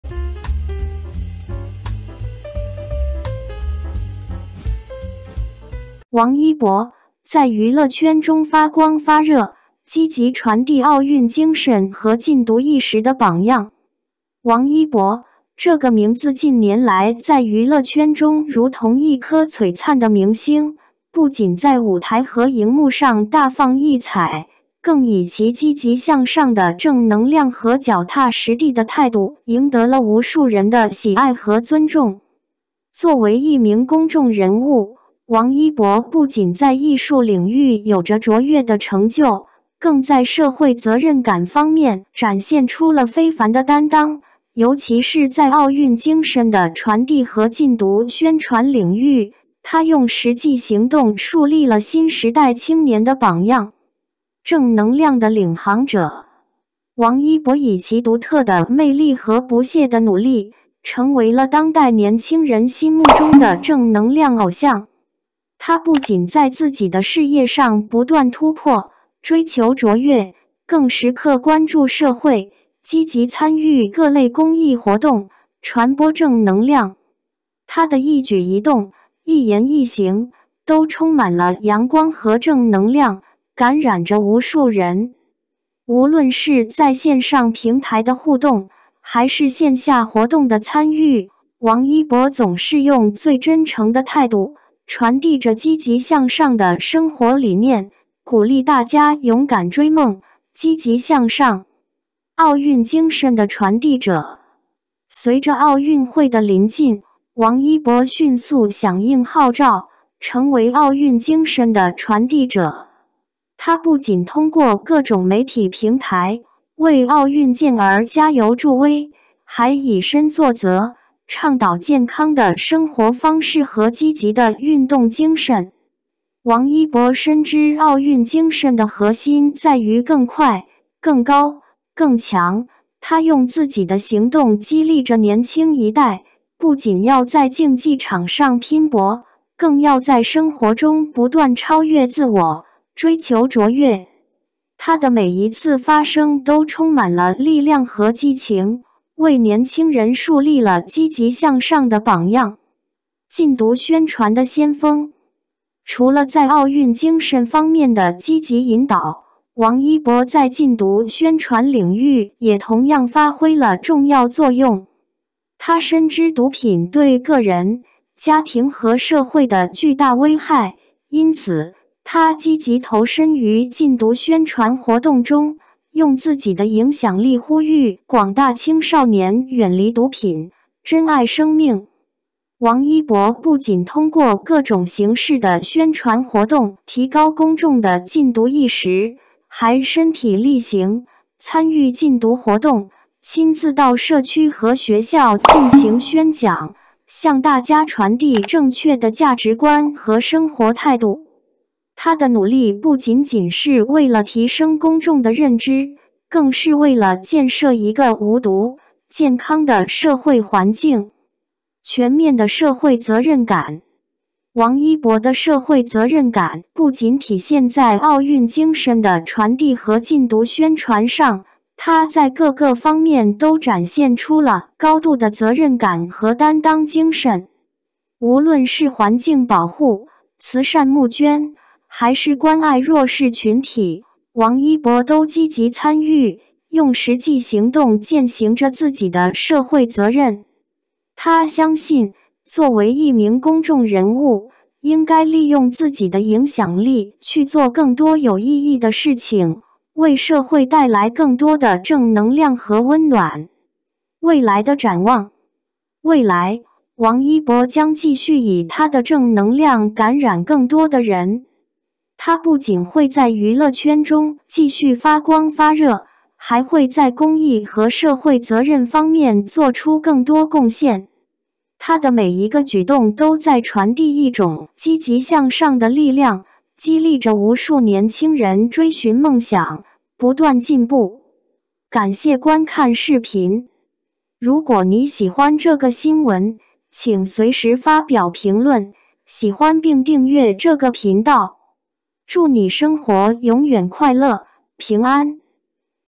王一博：在娱乐圈中发光发热，积极传递奥运精神和禁毒意识的榜样
他深知毒品对个人、家庭乃至社会的巨大危害，因此，他总是不遗余力地呼吁大家提高警惕，增强自我保护意识，共同营造一个无毒、健康的社会环境。王一博的禁毒宣传不仅仅停留在口头上，他更是身体力行地参与到各种禁毒活动中去，用实际行动践行着自己的承诺和信仰。